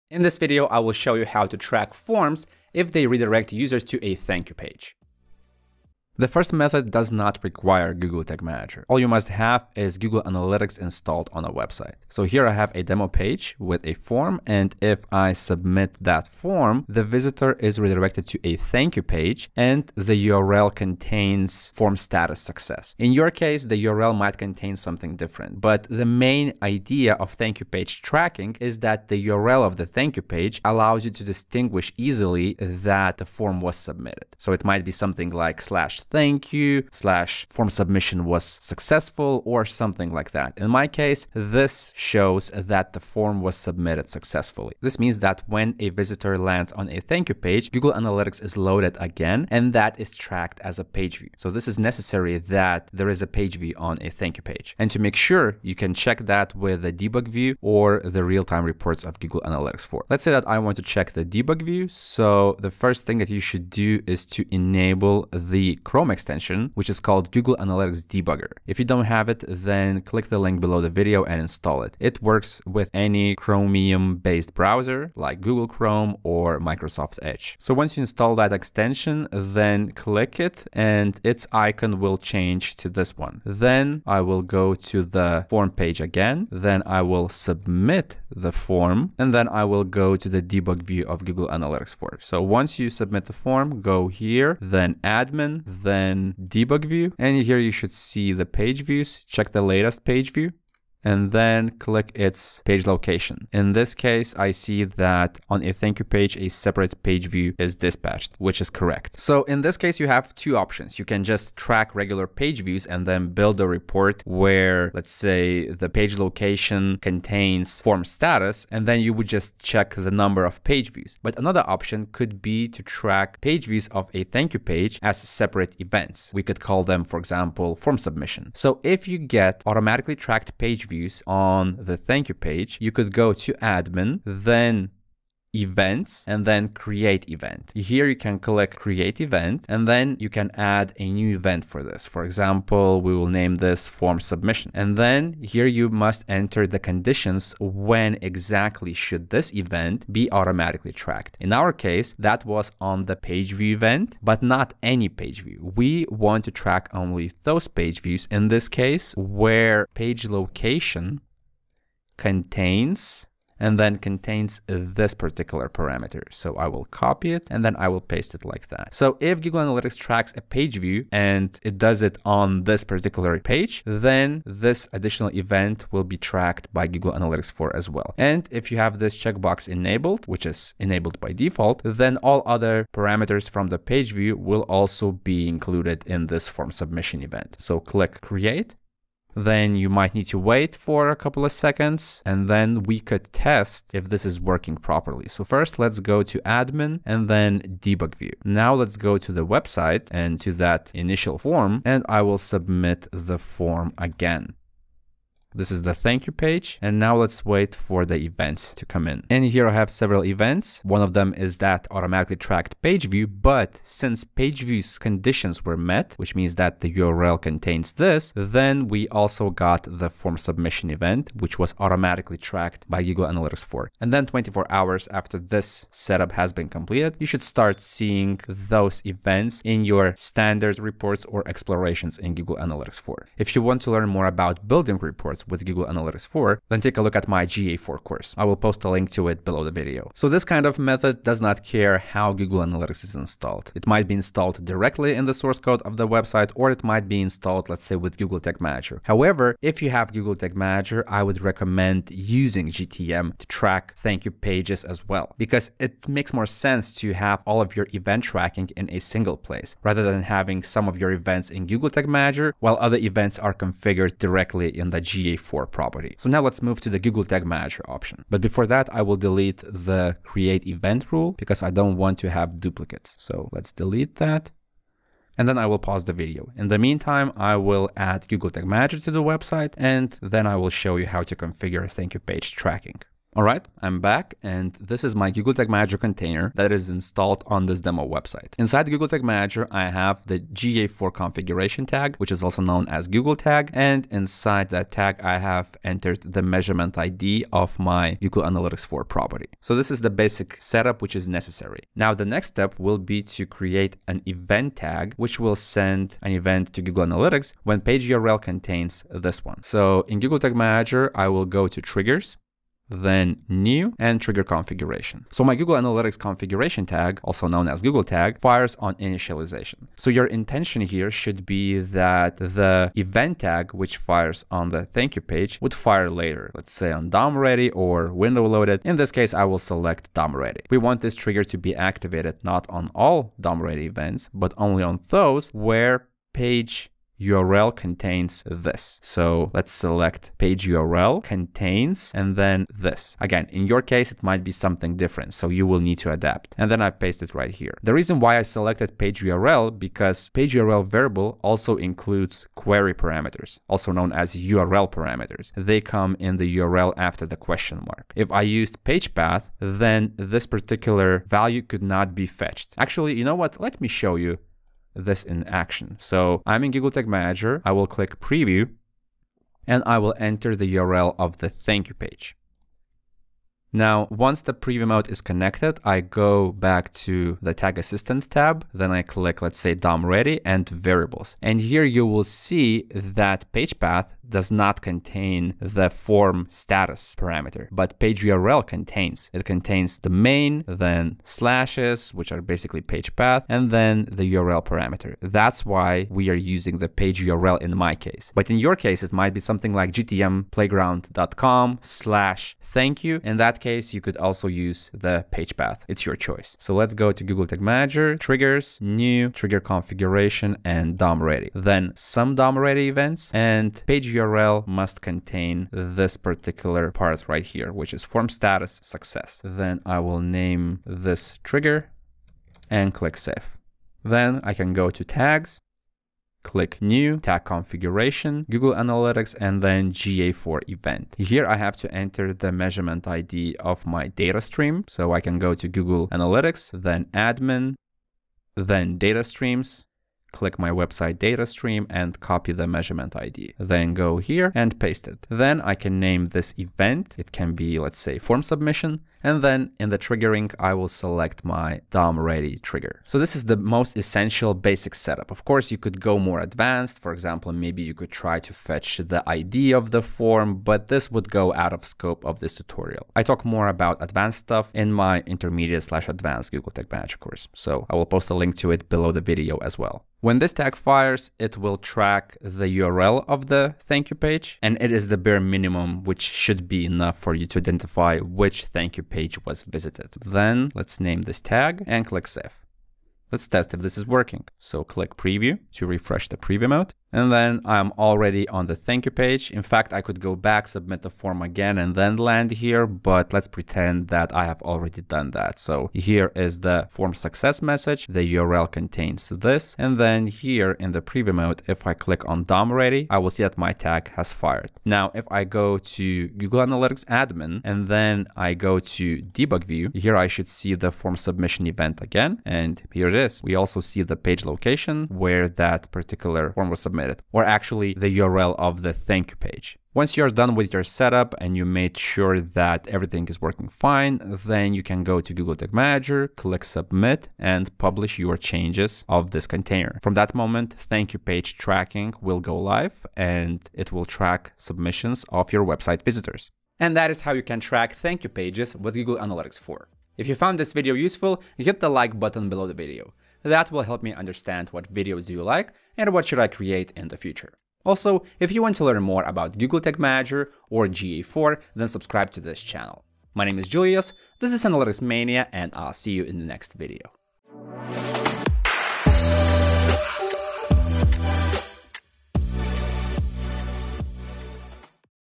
How to track thank you pages with Google Analytics 4 (and Google Tag Manager)
Learn how to track thank you pages with Google Analytics 4. In this video, you will learn two methods: one with Google Tag Manager and one without it.

=== LINKS ===